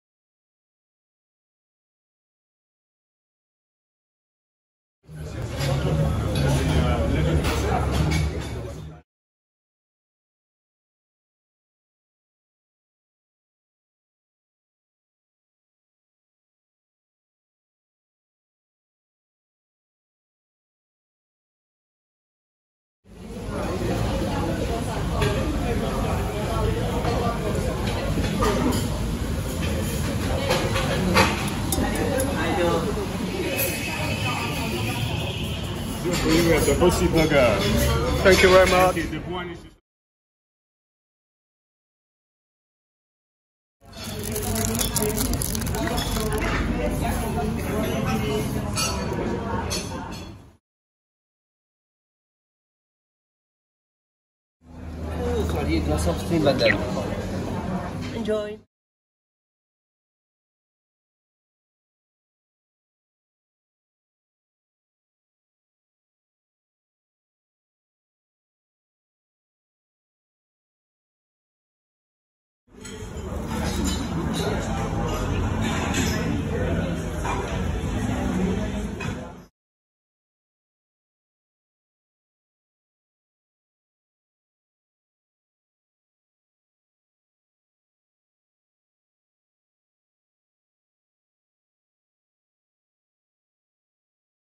Bibendum Oyster Bar, Michelin House, 81 Fulham Road, Chelsea, London SW3 6RD
Bosi 8oz Whopper Burger and two glasses of Zinny.